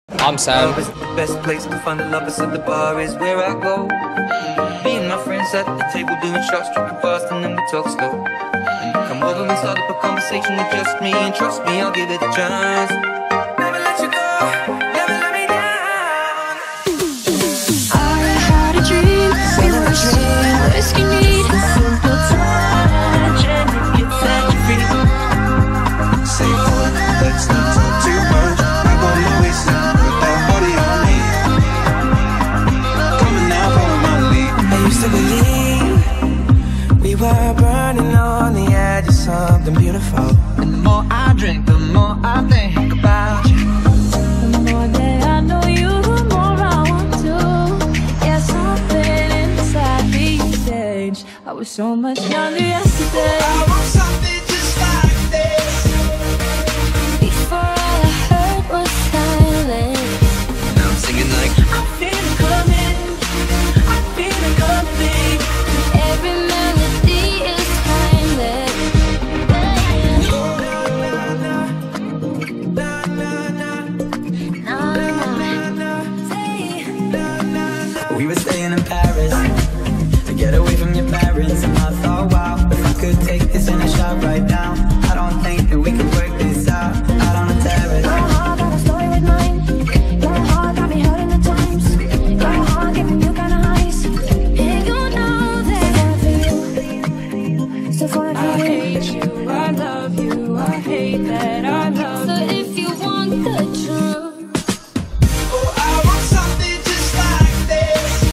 Happy 27th Birthday Sam Craske (Video Edit!)
A video edit for Sam from Diversity for his 27th birthday!! :)